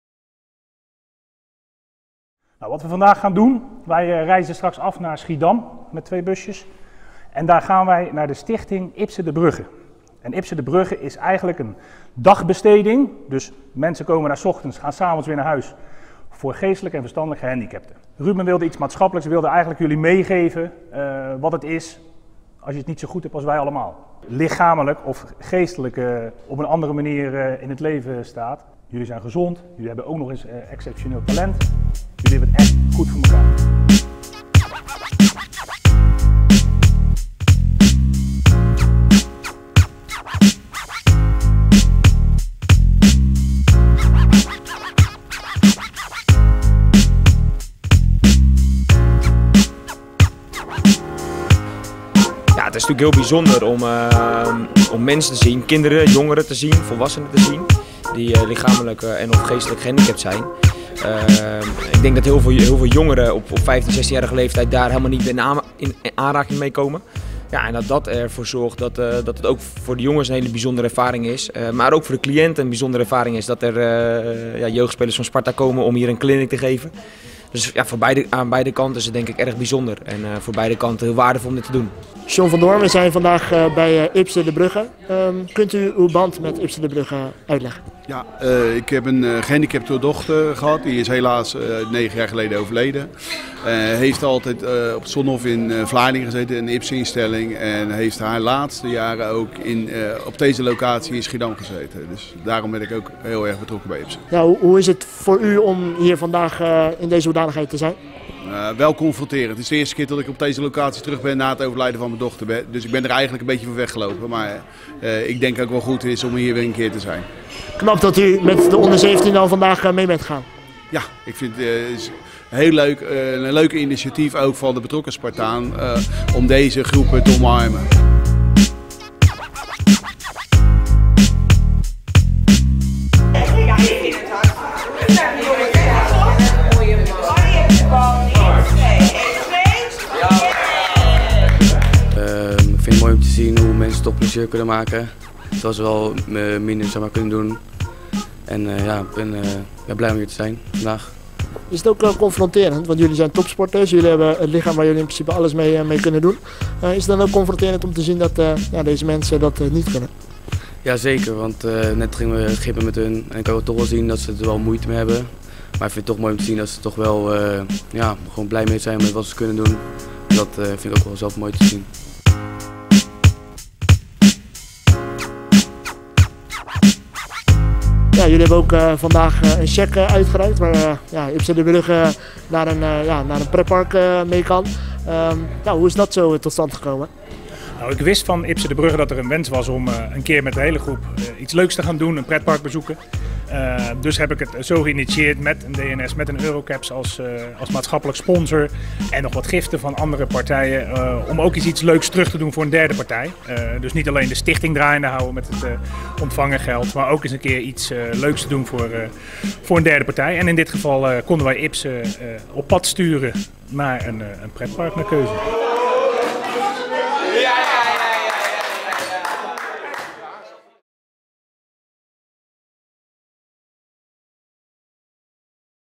Sparta O17 geeft clinic bij zorgcentrum Ipse de Bruggen: 'Een hele mooie middag'
Sparta Rotterdam O17 bezoekt zorgcentrum Ipse de Bruggen. Volgens trainer Ruben den Uil is het belangrijk dat jongens op deze leeftijd meer ervaren dan alleen op een voetbalveld staan.